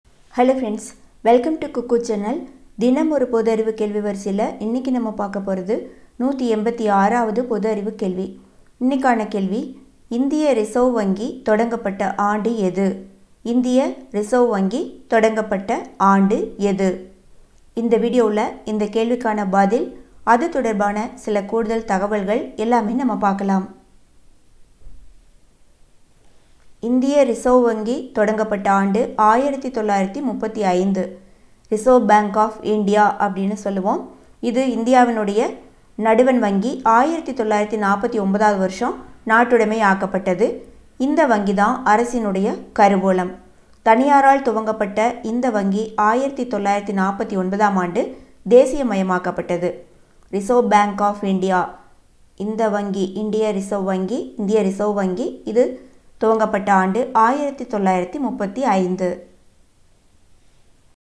பொது அறிவு வினா - 186
இந்திய ரிசர்வ் வங்கி தொடங்கப்பட்ட ஆண்டு எது?